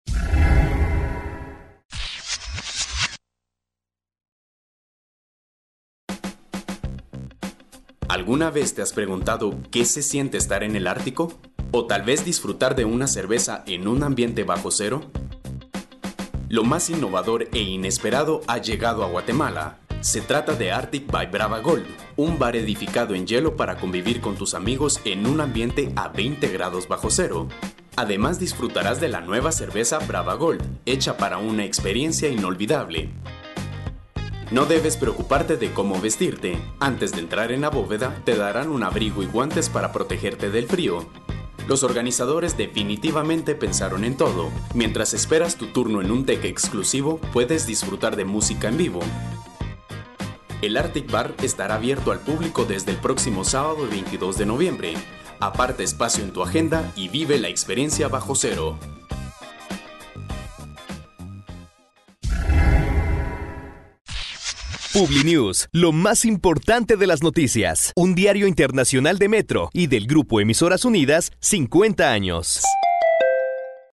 Conoce el primer bar de hielo en Guatemala
¿Alguna vez te has preguntado qué se siente estar en el Ártico? Lo más innovador e inesperado ha llegado a Guatemala. Se trata de Artick by Brahva Gold, un bar edificado en hielo para convivir con tus amigos en un ambiente a 20 grados bajo cero